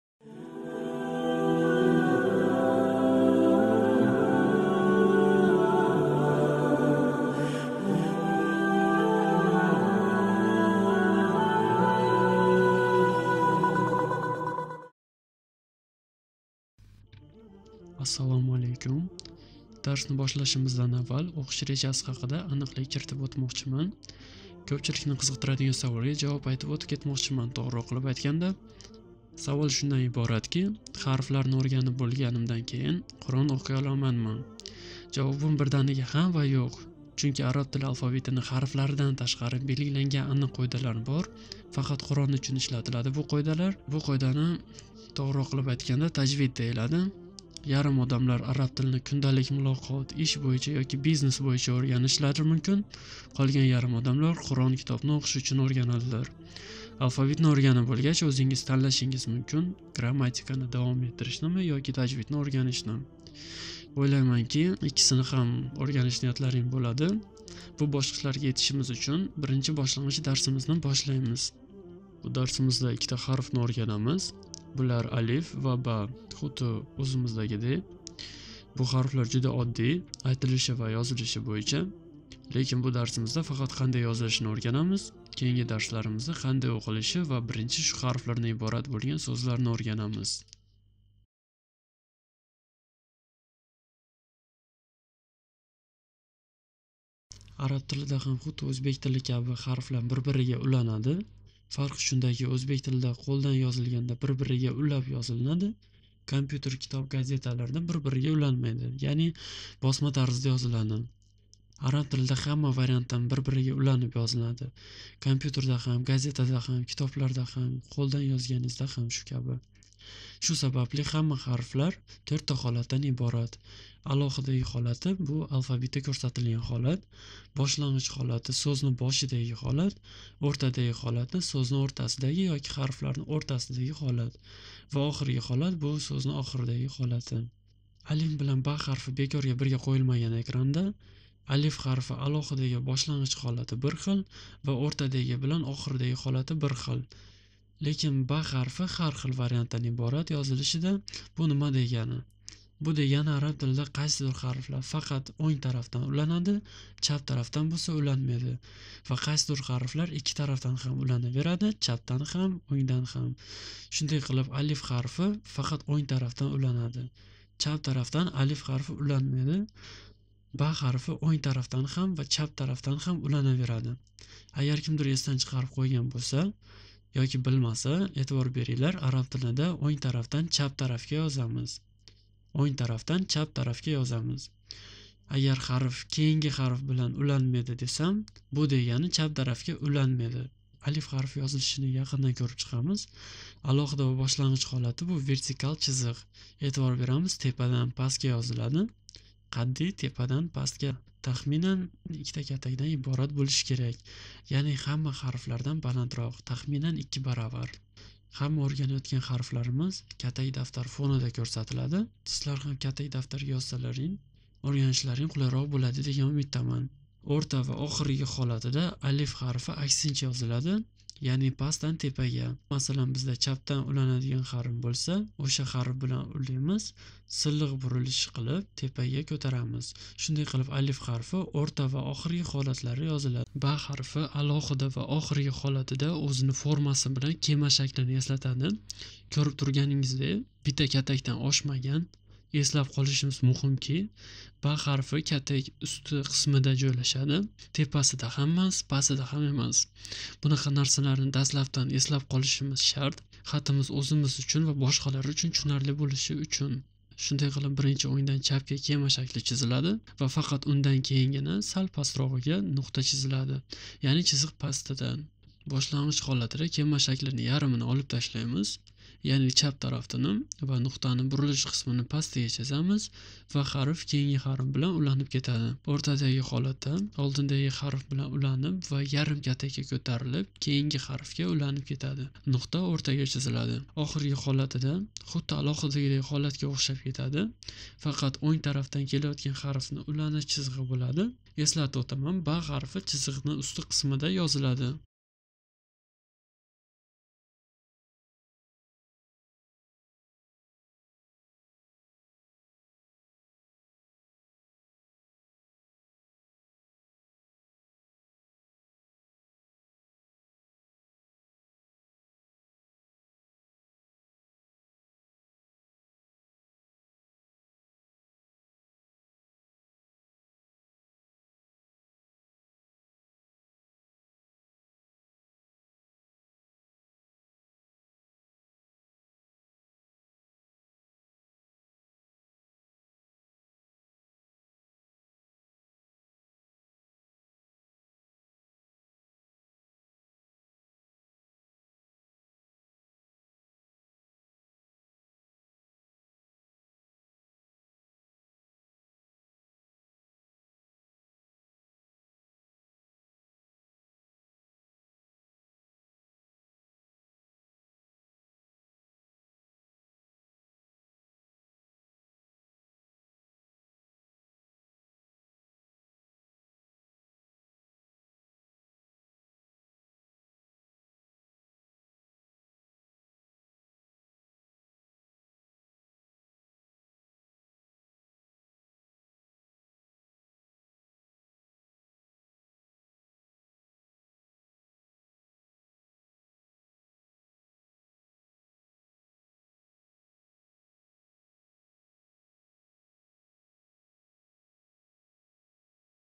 ARAB ALIFBOSI | ALIF ا va BA ب. Arab tili 0 dan - arab tilini o'rganish
ARAB ALIFBOSI | ALIF ا va BA ب. Arabi tili 0 dan - arab tilini o'rganish -

Tajvid qoidalari 1 dars. Tajvid fani, Qalqala, Iqlob, Ixfo, G'unna, Tajweed